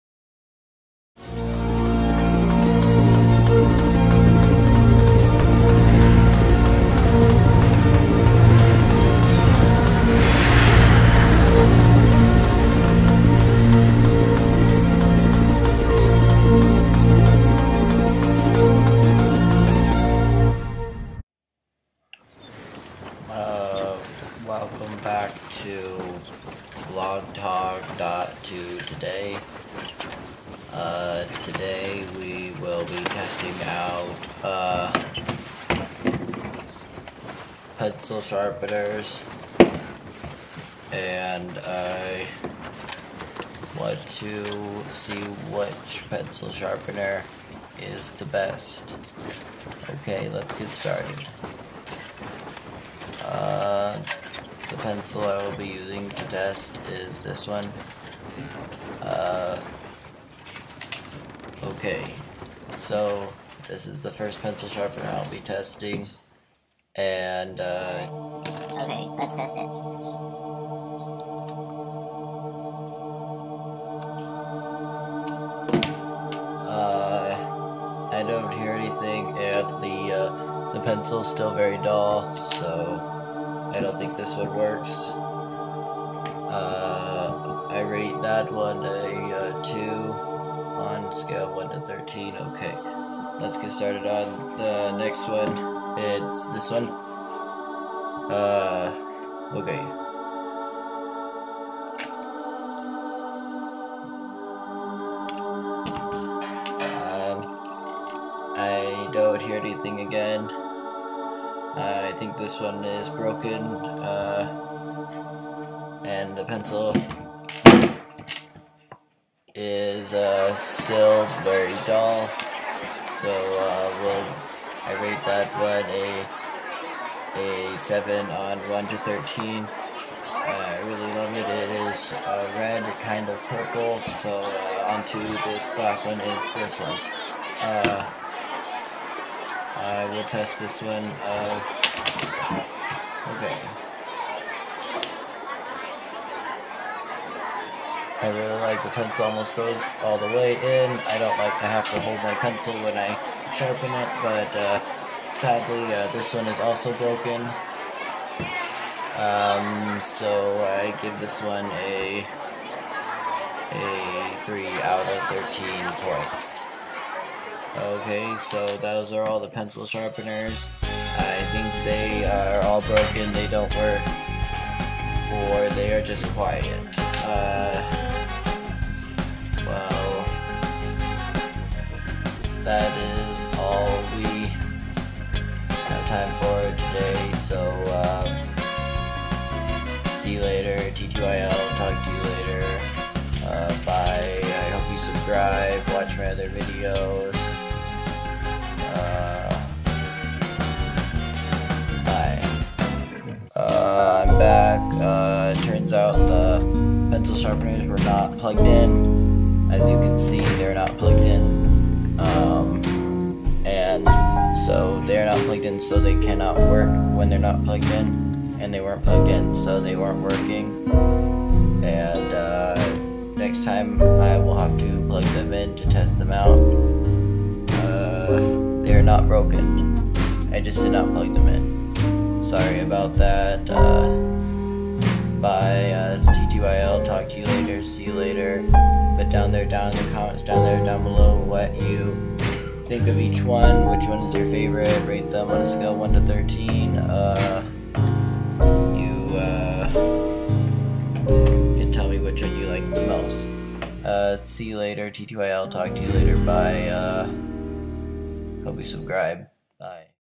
Testing pencil sharpeners
Vlog tog . 2 today episode 5
today i test out different pencil sharpeners.